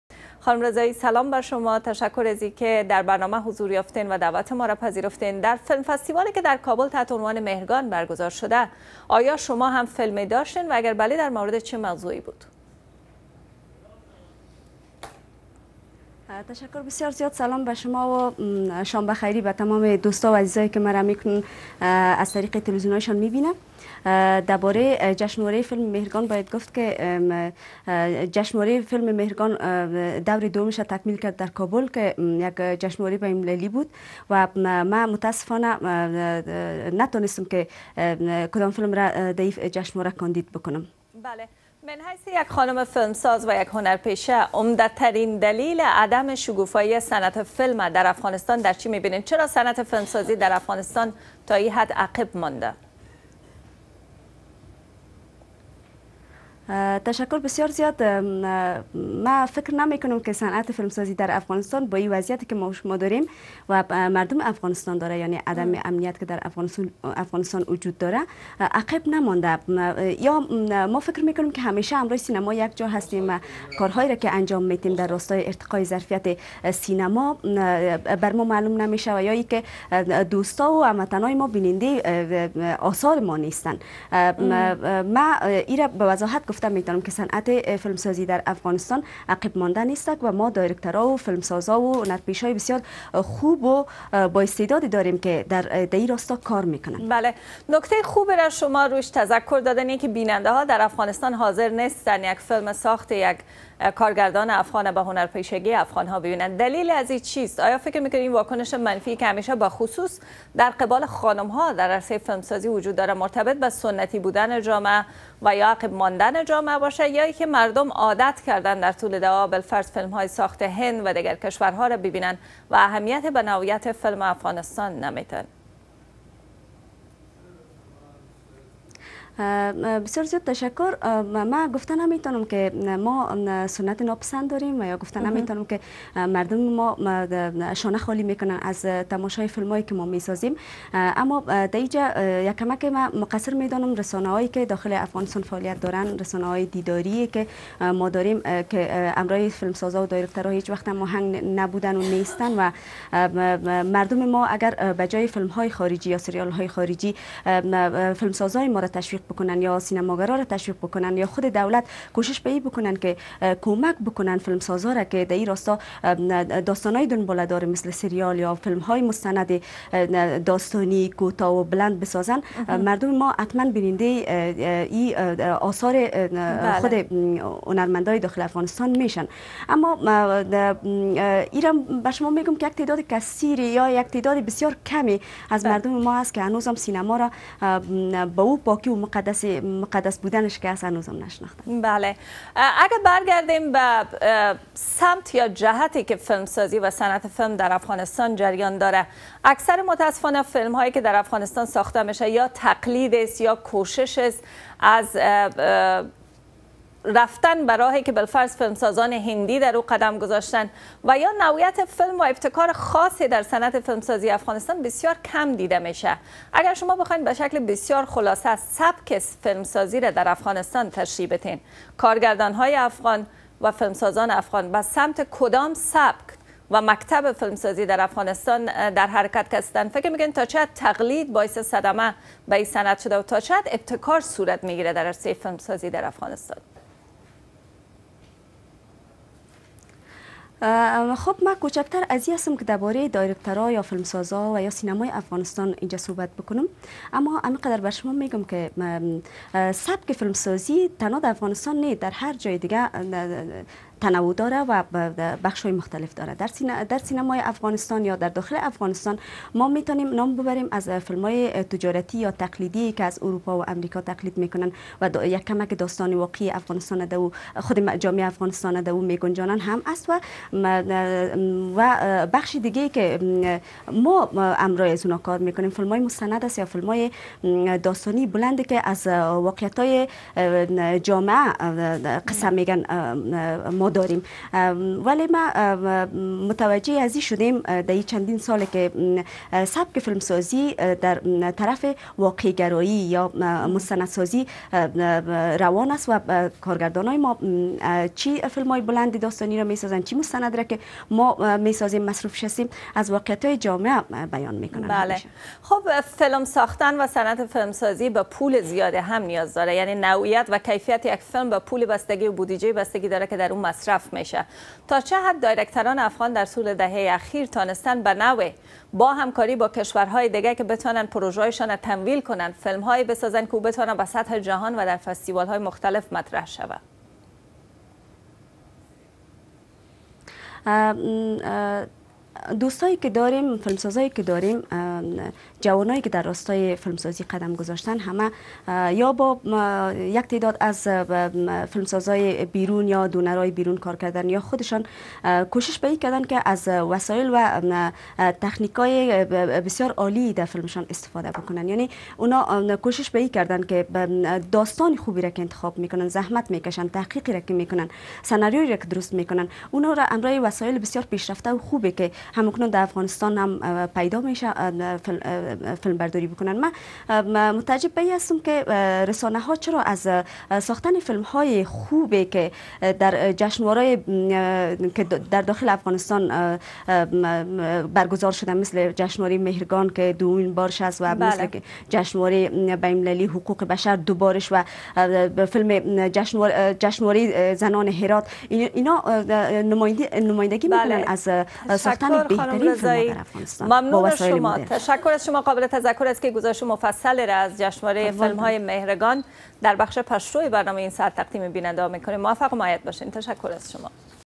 Rezai: Afghan film industry is thriving
Interview with Aqila Rezai, actress and director
Oct.8.2015